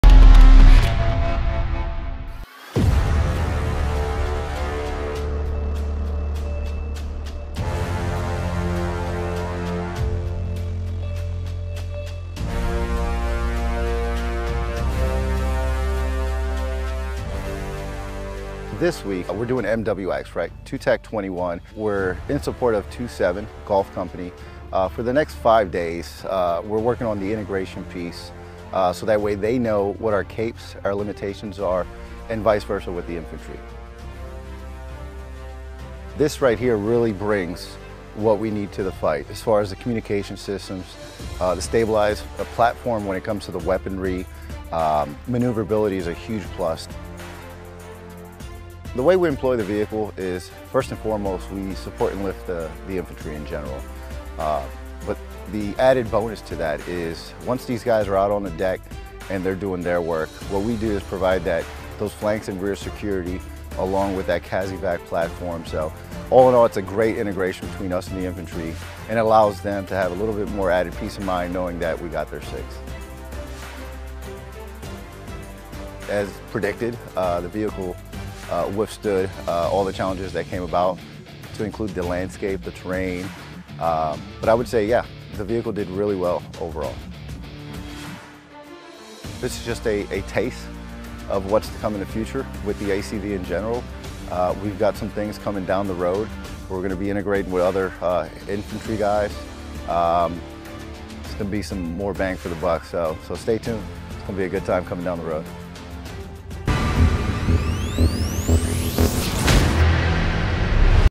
U.S. Marine Corps shows off capabilities of its new Amphibious Combat Vehicles
If you would like to show your support for what we are doing, here's where to do it